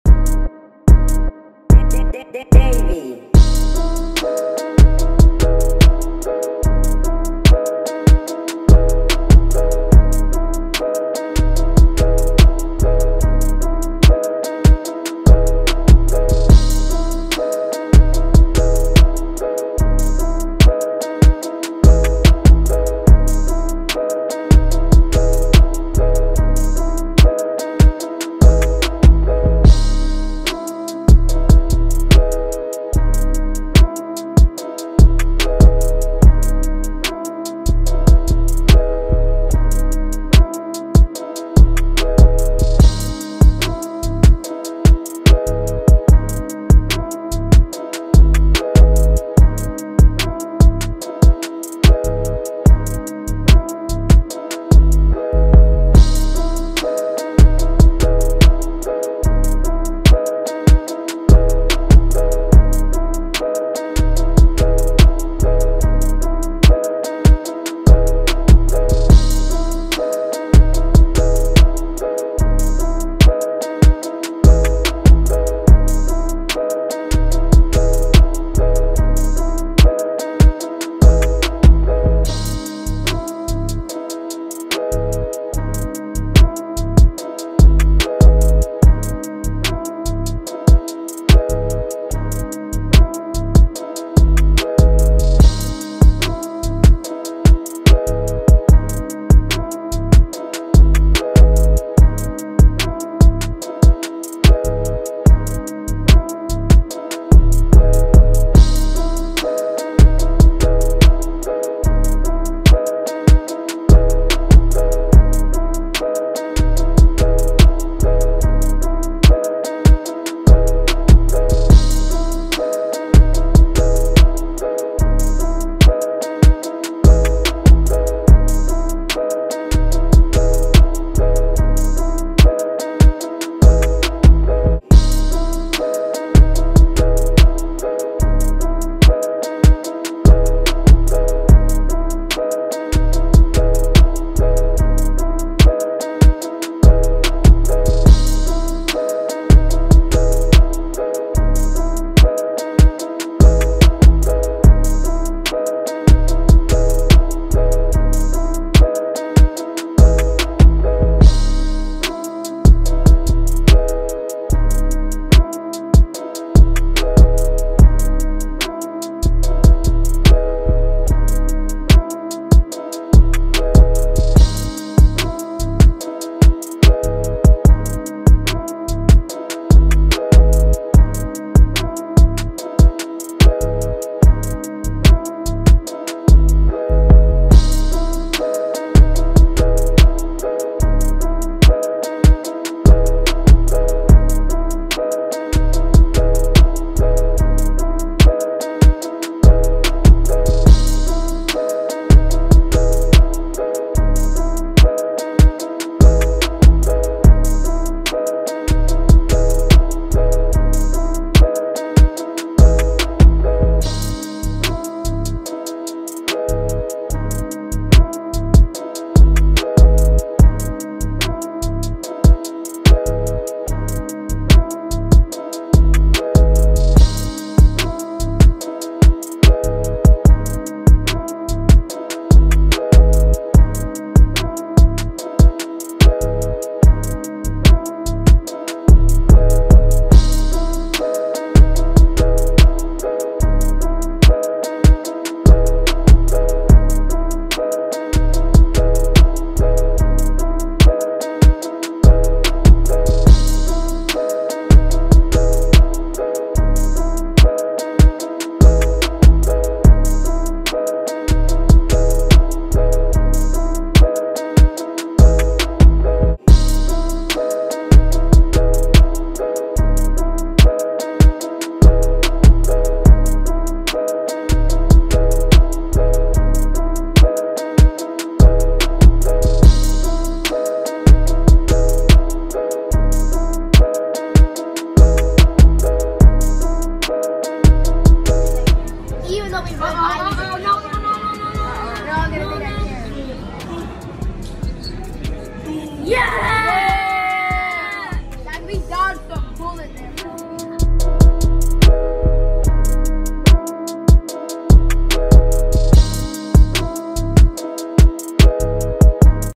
3v3 Indoor Soccer Tournament Highlights | The Fantastic Four | Denzel Morin 11 years old
The Fantastic Four team (West Michigan Storm) played in the Michigan Rangers 3v3 indoor soccer tournament in the U12 division in Jenison, Michigan on November 26, 2022.

Music (Prod. Davy)